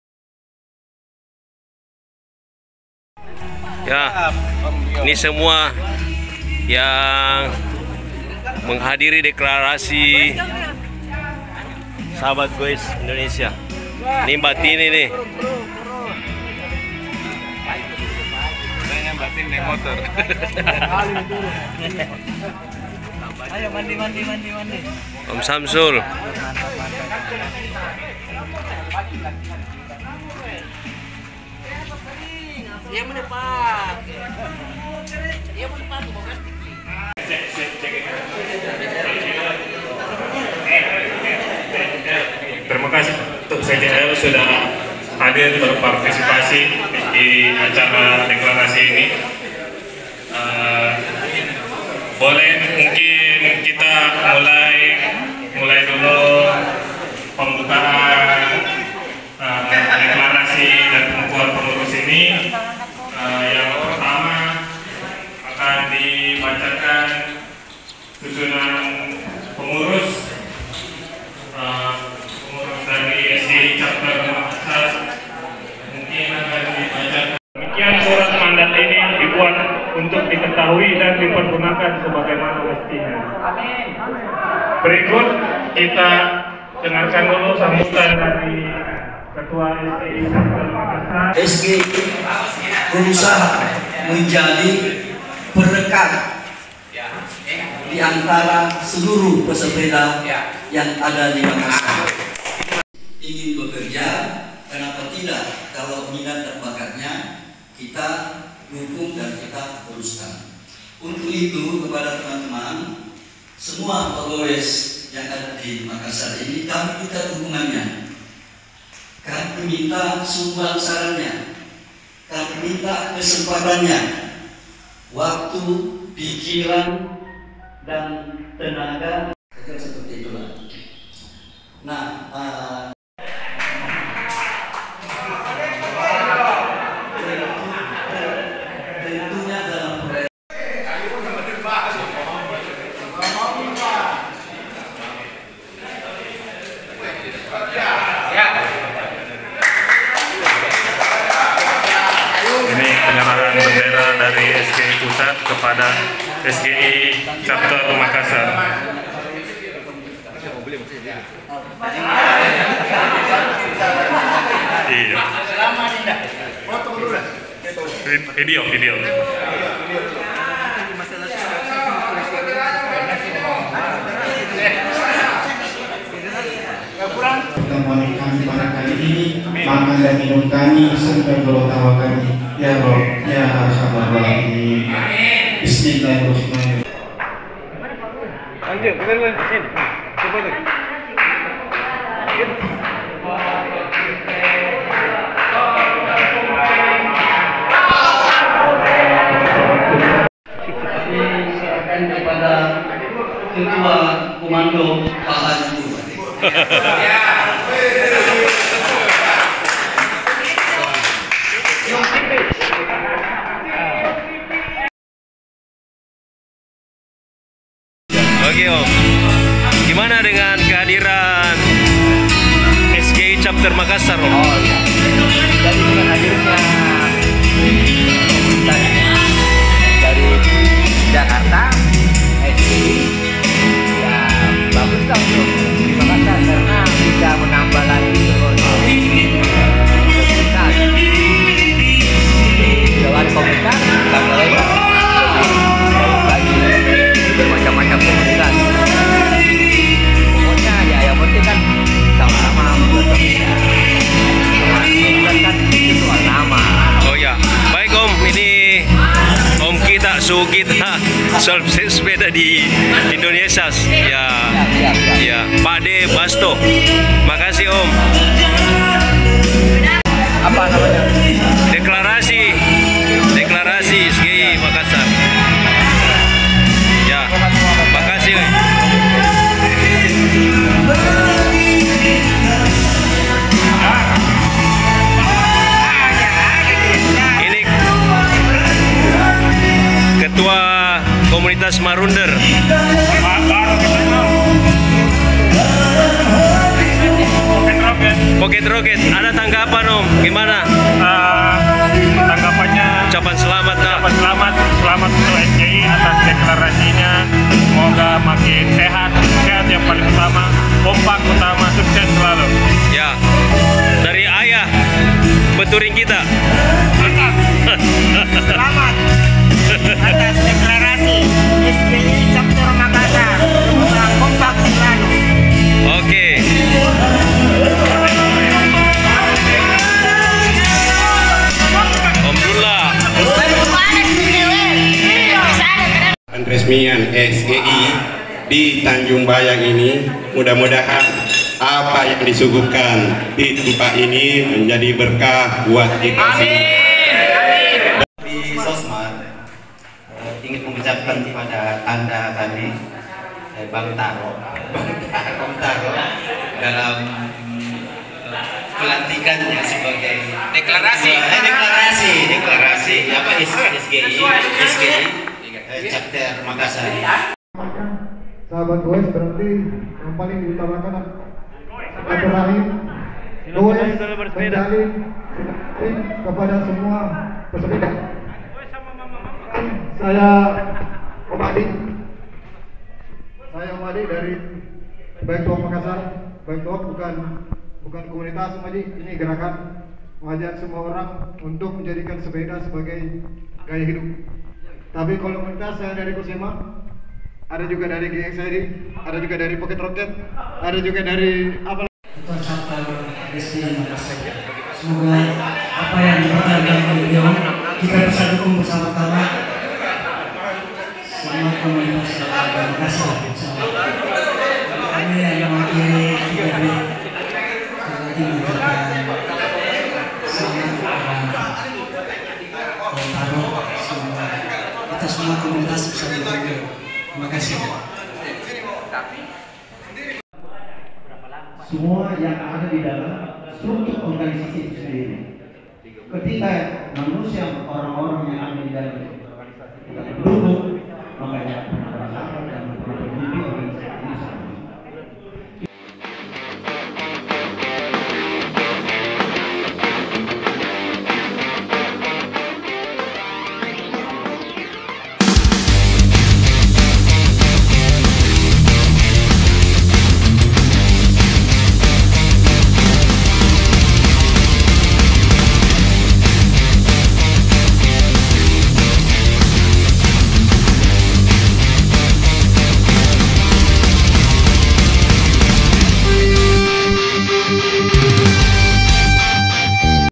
MERIAH DEKLARASI SGI MAKASAR - KOMUNITAS SEMUA JENIS SEPEDA
Komunitas Sahabat Gowes Indonesia  Chapter Makassar telah resmi dideklarasikan pada 13 Agustus 2022. Komunitas ini adalah salah satu dari Chapter yang sudah ada sebelumnya di kota-kota lain di Indonesia.